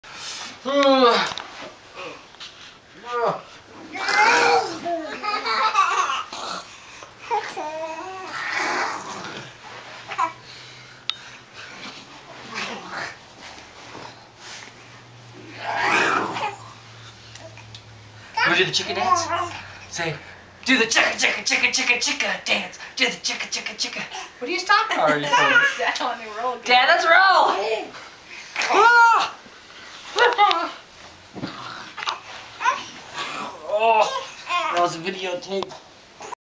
The David Roller Coaster Part 1
David did summersaults with his daughter and she loved it!